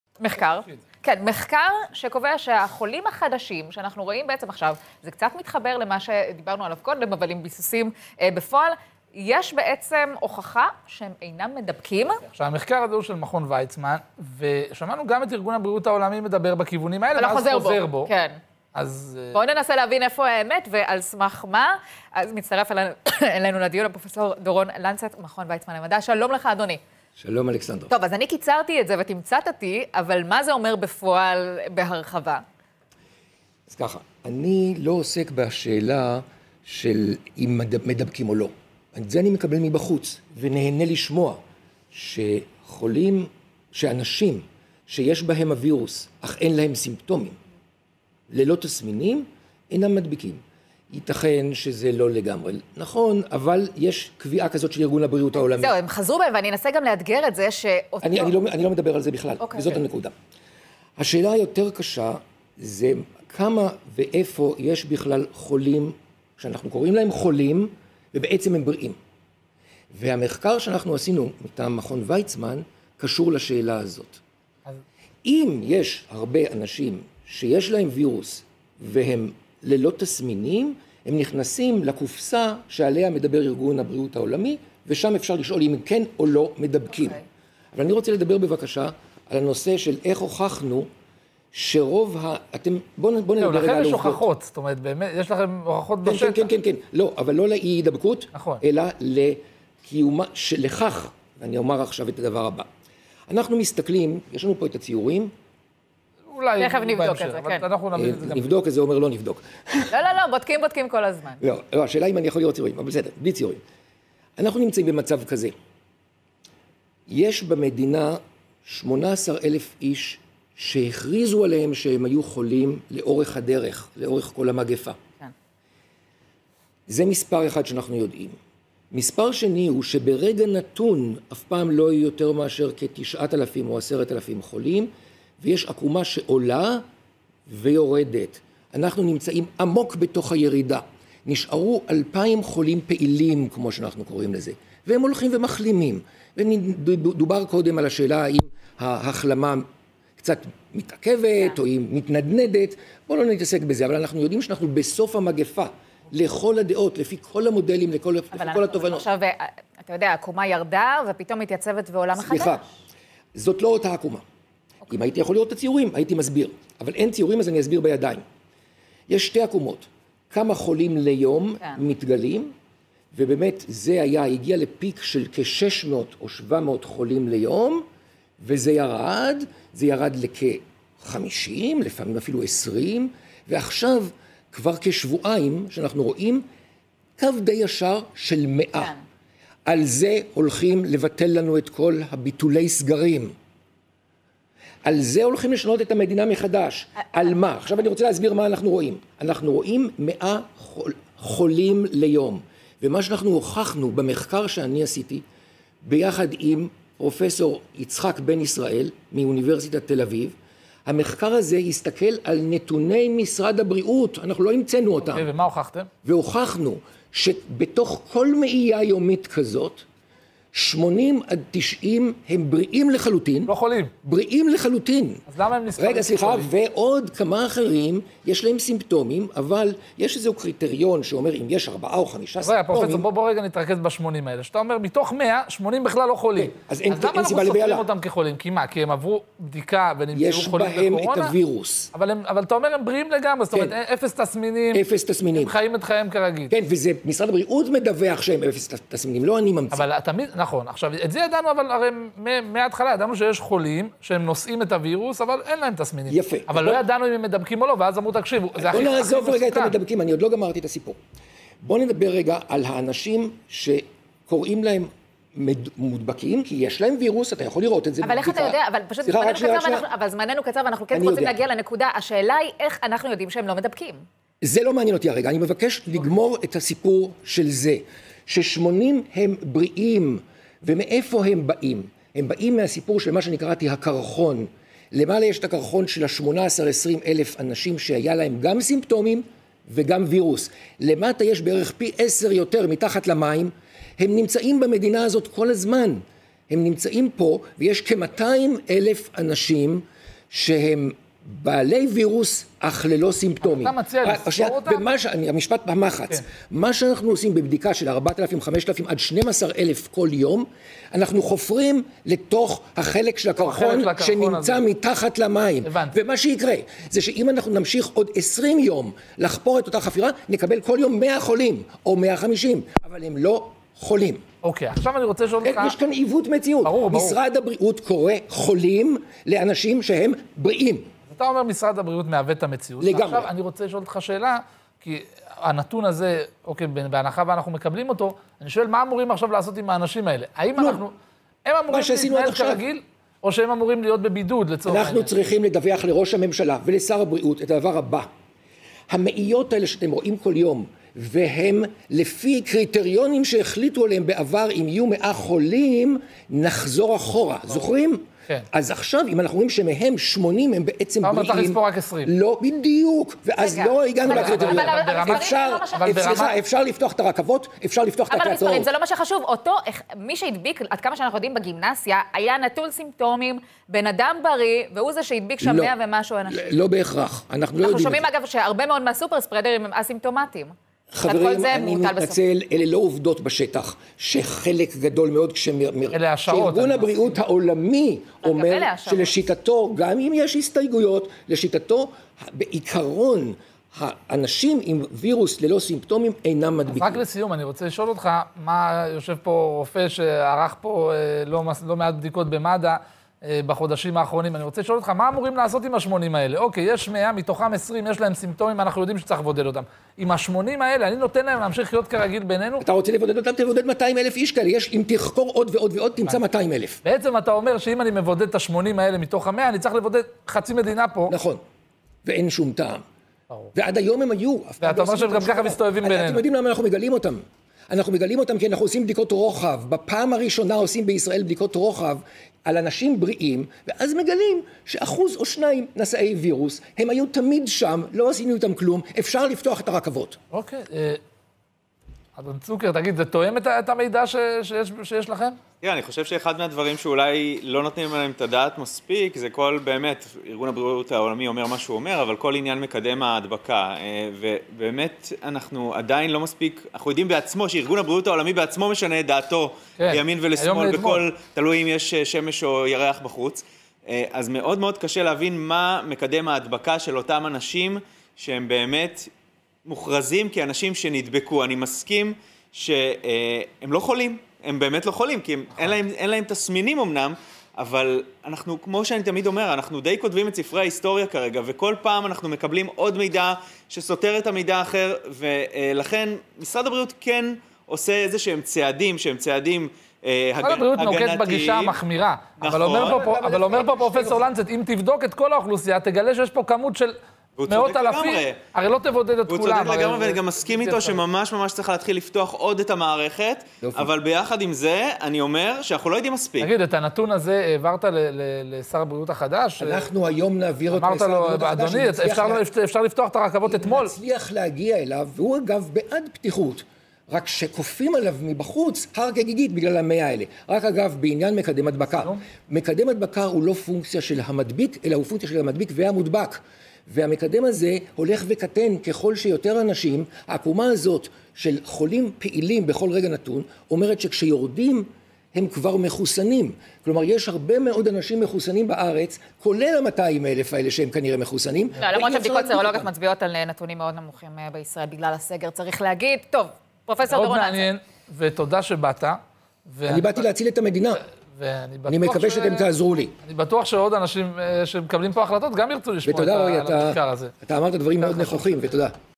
מכון ויצמן: "החולים החדשים כלל לא מדבקים"
זינוק בחולי הקורונה? אחוז גבוה מאוד של "החולים חדשים" הוא אנשים חסרי תסמינים - שאינו מהווה "גל שני". פרופ' דורון לנצט ממכון ויצמן למדע ופרופ' יצחק בן ישראל מאוניברסיטת תל-אביב מסבירים